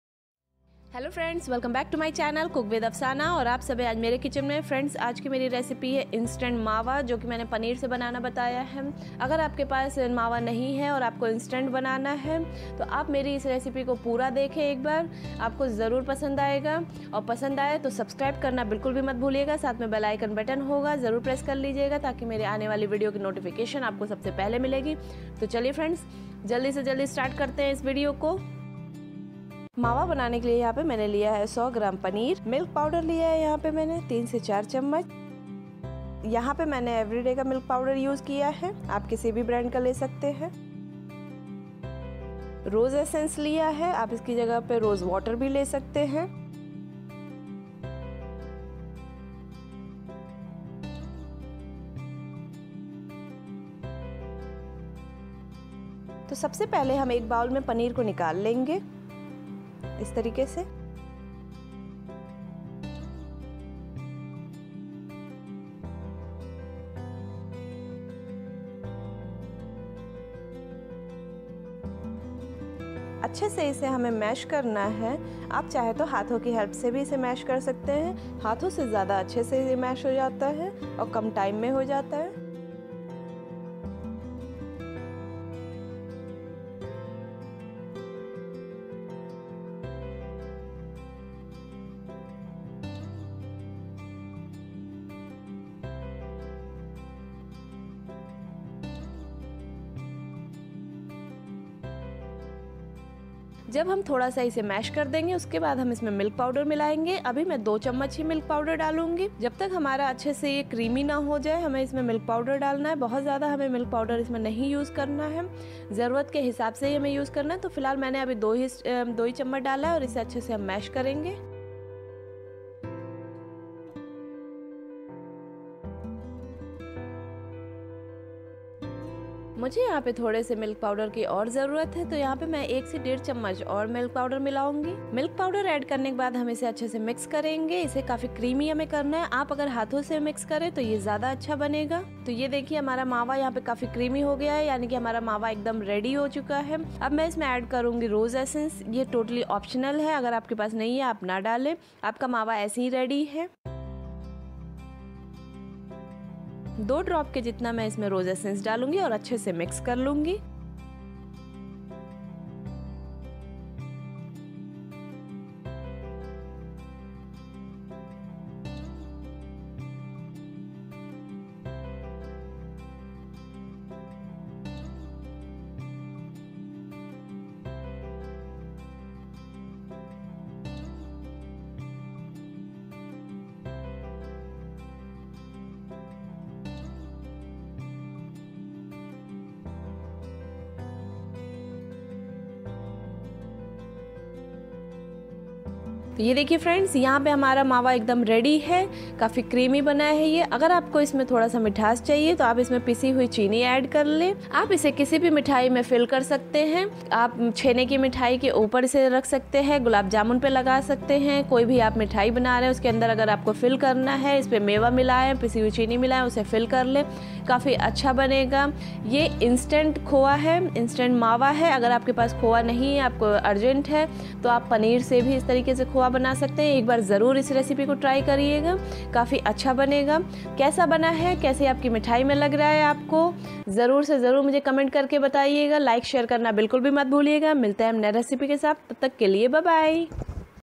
Instant Mawa or Khoya | १ मिनट में पनीर से बनाएं मावा (खोया )
Hi friends, In this video I have shown how to make instant mawa or khoya in 1 minute by using Paneer.